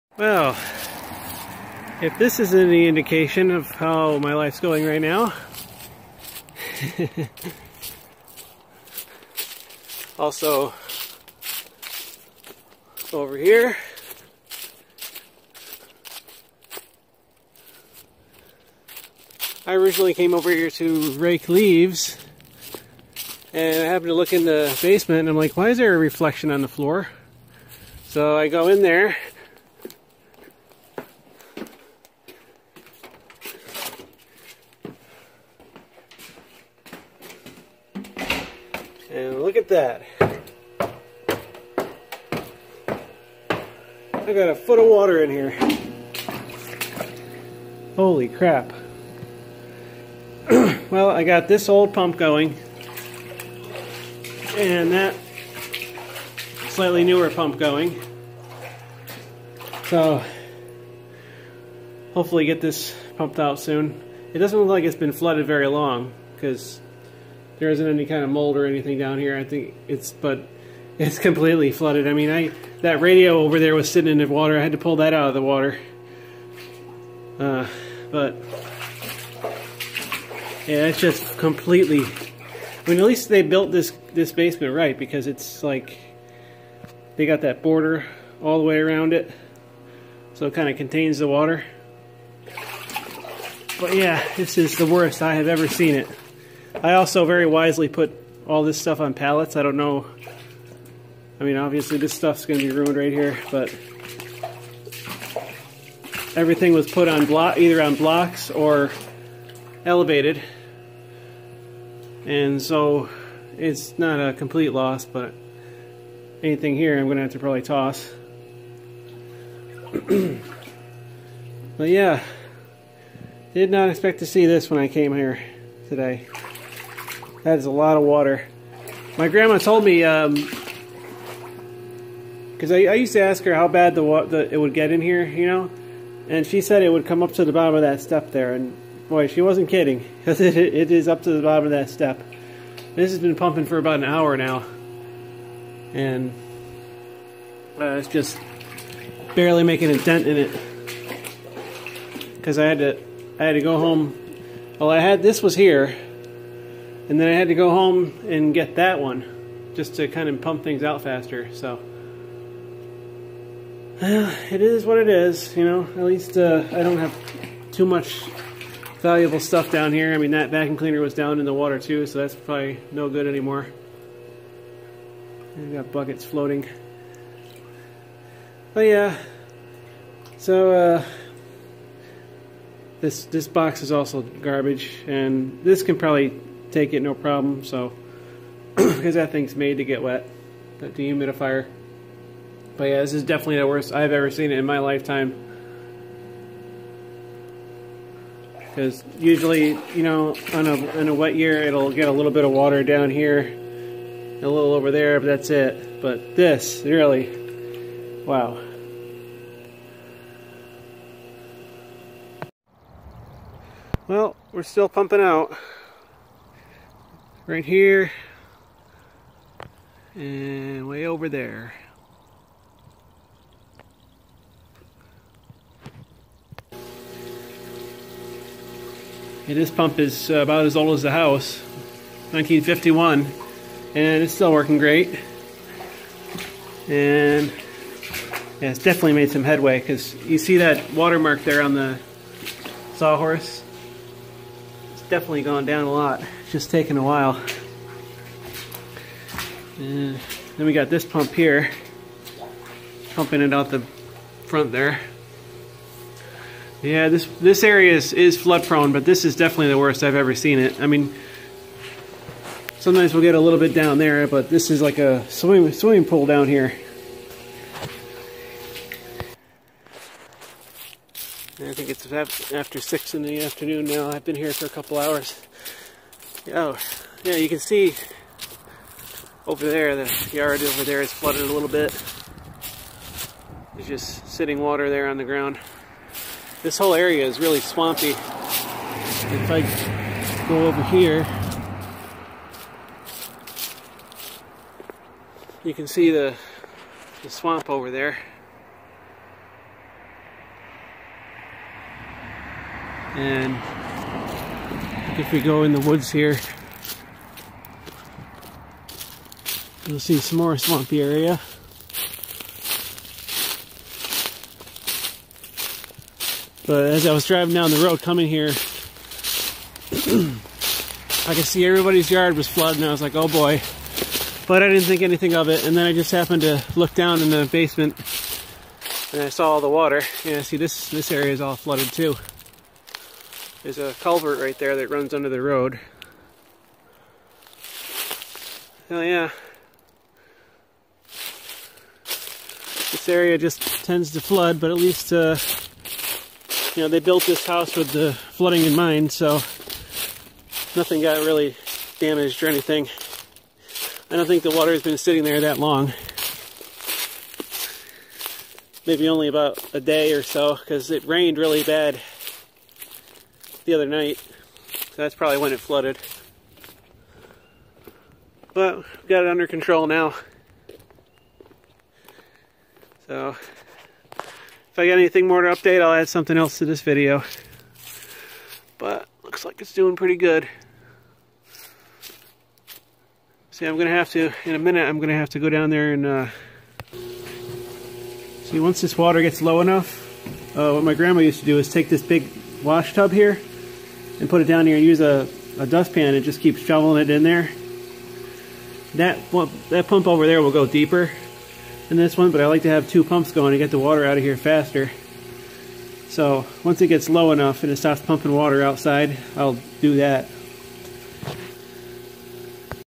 My basement is flooded!
Found out after making this video the water is still coming in. But I was able to pump most of it out today.